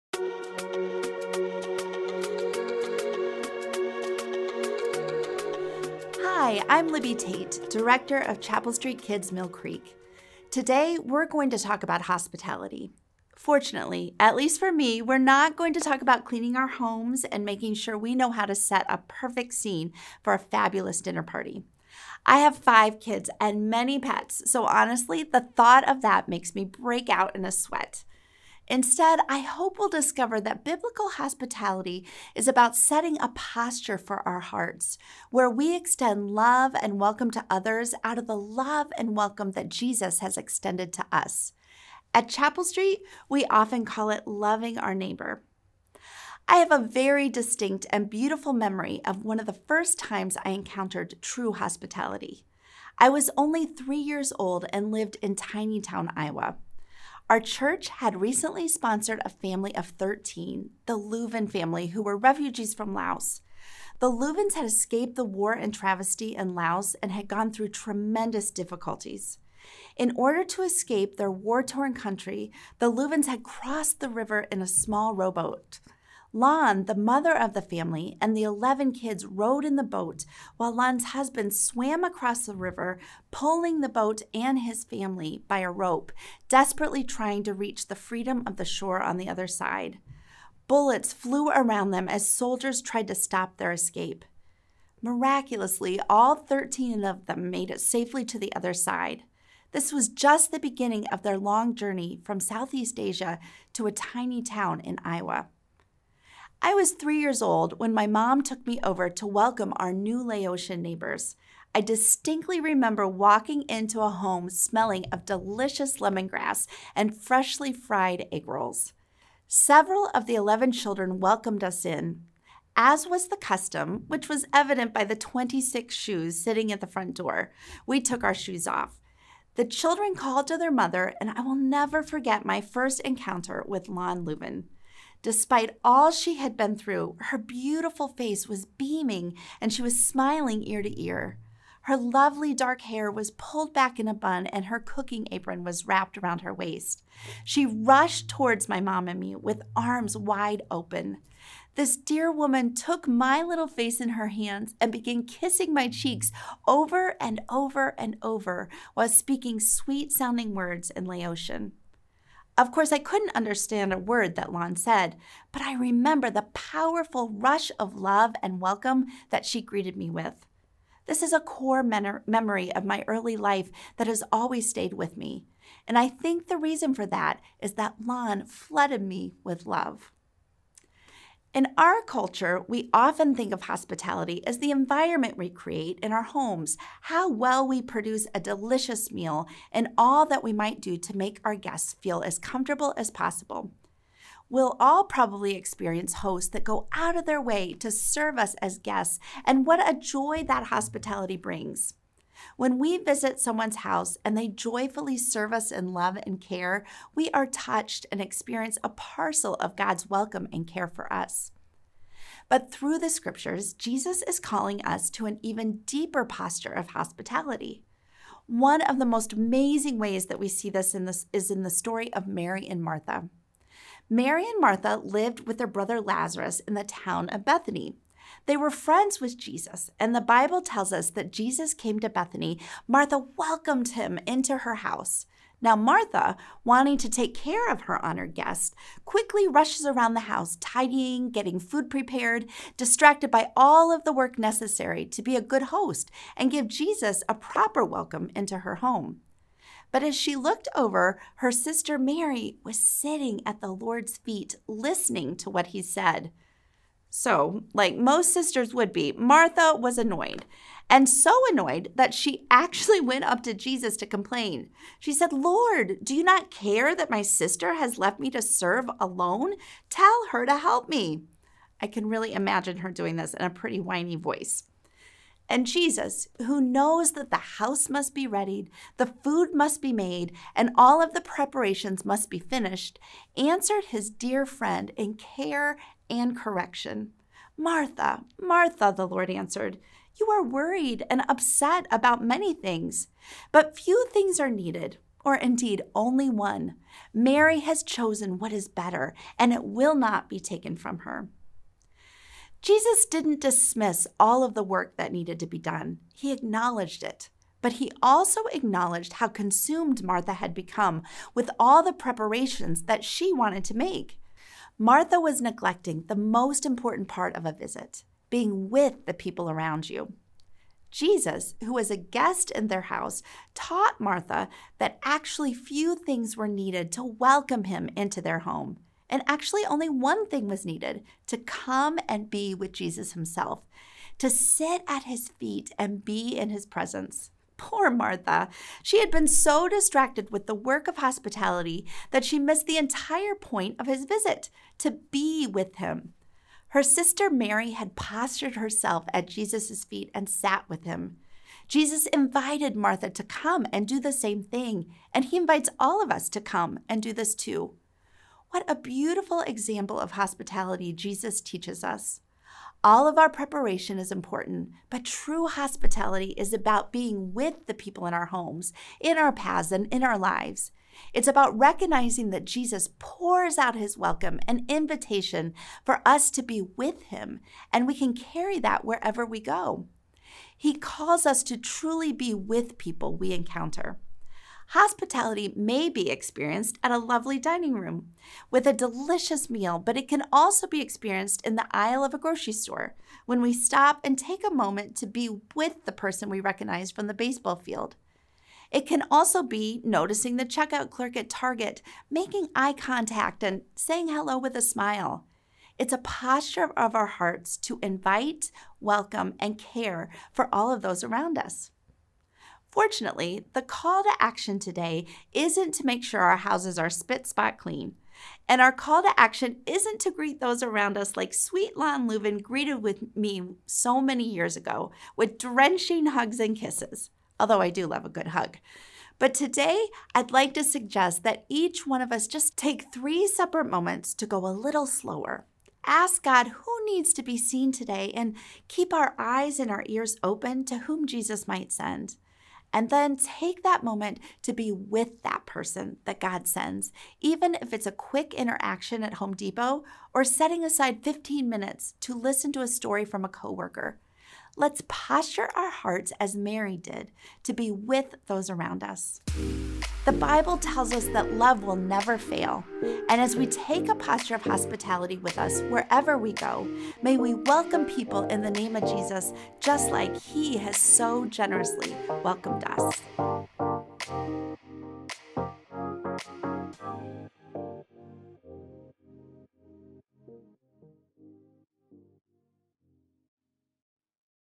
What is biblical hospitality?
What is hospitality, and how do we practice it?

What are the first three attributes that come to your mind when you think of Jesus? Take a moment to write or think about your answers.

Which three attributes did you choose? Loving? Gracious? Kind? Merciful? Compassionate? Holy? Just? Humble? Gentle? Forgiving? Patient? Self-controlled? Obedient? Honest? Prayerful?  Did you pick any attributes outside of this list?

Where does Christ’s hospitality rank on your list? Perhaps you have rarely or never thought about hospitality as an attribute of Christ. After all, how could Jesus be hospitable if he never even owned a house?

Hospitality includes much more than cleaning up your home before having guests over for dinner (although that’s certainly a wonderful way to love and serve others!). The beautiful attribute of hospitality is something anyone can demonstrate—and it wonderfully reflects the heart of Christ.

In today’s video, the Director of Chapelstreet Kids for our Mill Creek campus, Libby Tate explains the true meaning of hospitality, and how we can reflect this beautiful, Christlike quality in our lives.

This is Day 18 of our 20 Days on Following Jesus series.

A Prayer for Today
Loving Father, thank you for calling me to yourself when I was far from you. Thank you for adopting me, making me your child, and welcoming me into your family. Thank you for rescuing me from the domain of darkness and transferring me into the kingdom of your beloved Son. Thank you for giving me an eternal family of brothers and sisters in Christ. Thank you for taking up residence in my heart, dwelling with me in this life, and preparing for me an eternal home full of joy and peace. You are so good to me. Help me to reflect your love and welcoming heart to those around me today. Bring many more people into your family, including my loved ones. I love you, Lord. Amen.

BIBLE VERSES REFERENCED
Luke 10:40–42: “But Martha was distracted by all the preparations that had to be made. She came to him and asked, ‘Lord, don’t you care that my sister has left me to do the work by myself? Tell her to help me!’ ‘Martha, Martha,’ the Lord answered, ‘you are worried and upset about many things, but few things are needed—or indeed only one. Mary has chosen what is better, and it will not be taken away from her.’”

QUOTES
“Biblical hospitality is about setting a posture for our hearts, where we extend love and welcome to others out of the love and welcome that Jesus has extended to us.”

ADDITIONAL VERSES TO EXPLORE
Matthew 25:34–46
Luke 10:25–42
Luke 14:12–14
Romans 15:7